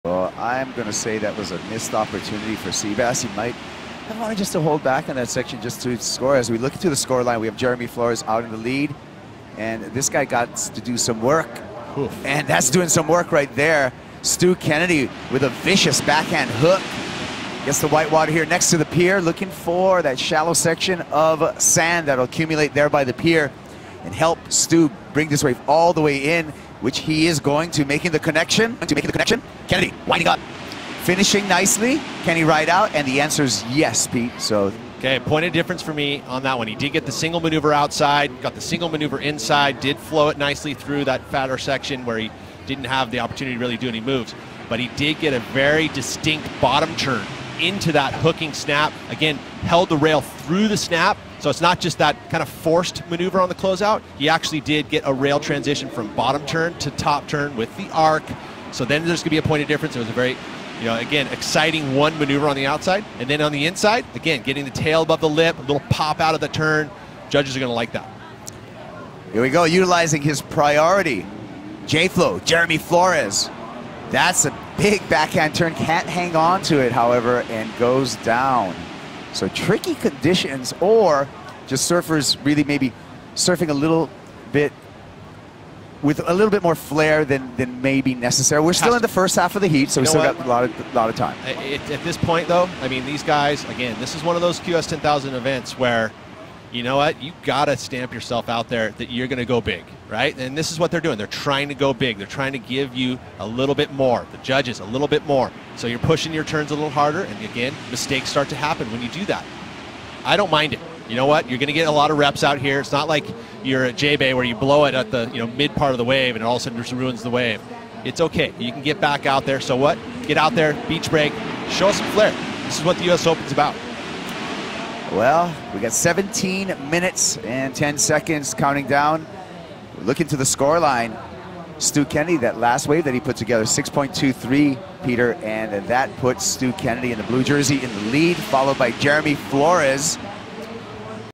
Stuart Kennedy's Viscous Backhand
Stuart Kennedy earns an 6.23 to take the lead over Jeremy Flores in Round Three Heat 3.